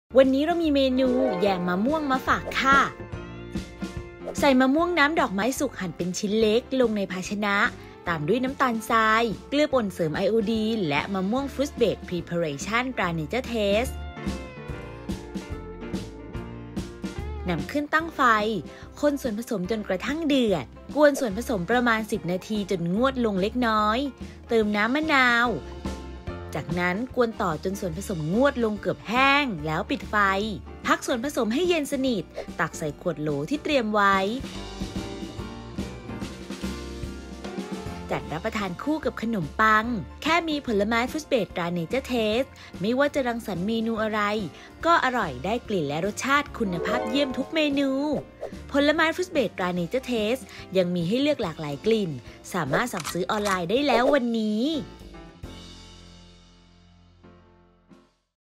แยมมะม่วง สูตรเด็ด ทำง่าย สไตล์ "อ.ยิ่งศักดิ์"
แยมมะม่วง  (ผลิตภัณฑ์: มะม่วงฟรุ๊ตเบส พรีเพอเรชั่น ตราเนเจอร์เทส)

มะม่วงน้ำดอกไม้สุกหั่นเป็นชิ้นเล็ก 500 กรัม
มะม่วงฟรุ๊ตเบส พรีเพอเรชั่น ตราเนเจอร์เทส 150 มิลลิลิตร
น้ำมะนาว    2 ช้อนชา
ขนมปัง สำหรับรับประทานคู่

1. ใส่มะม่วงน้ำดอกไม้สุกหั่นเป็นชิ้นเล็กลงในภาชนะ ตามด้วยน้ำตาลทราย เกลือป่นเสริมไอโอดีน                                   และมะม่วงฟรุ๊ตเบส พรีเพอเรชั่น ตราเนเจอร์เทส นำขึ้นตั้งไฟ คนส่วนผสมจนกระทั่งเดือด
2. กวนส่วนผสมประมาณ 10 นาที จนงวดลงเล็กน้อย เติมน้ำมะนาว จากนั้นกวนต่อจนส่วนผสมงวดลงเกือบแห้ง ปิดไฟ
3. พักส่วนผสมให้เย็นสนิท ตักใส่ขวดโหลที่เตรียมไว้ จัดรับประทานคู่กับขนมปัง

▲Line @yingsakfood